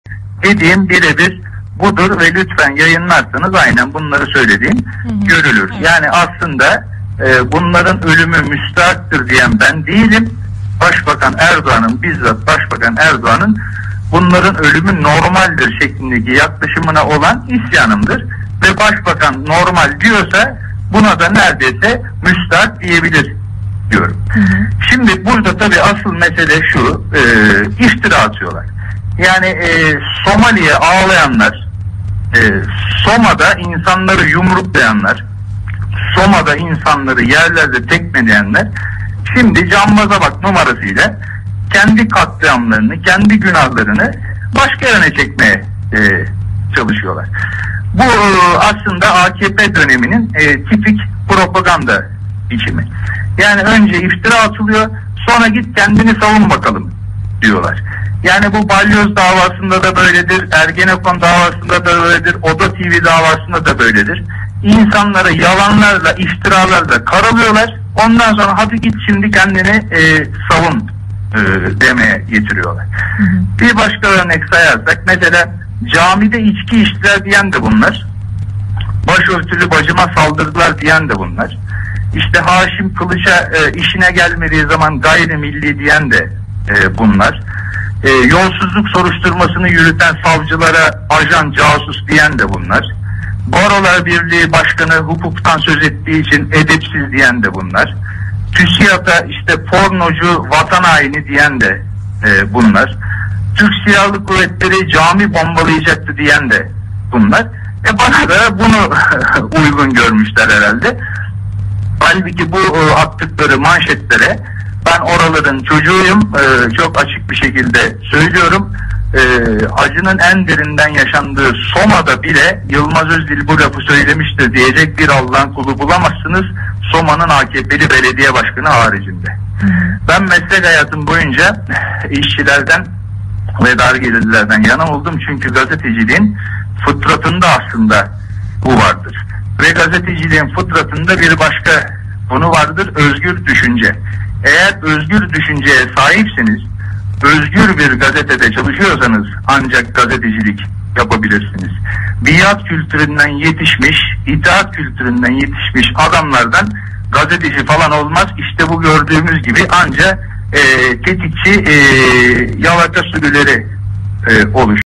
Yılmaz Özdil'den yeni 'Soma' açıklaması
Yılmaz Özdil Halk TV yayınına bağlanarak yeni açıklamlarda bulundu. İşte Özdil'in o açıklamaları..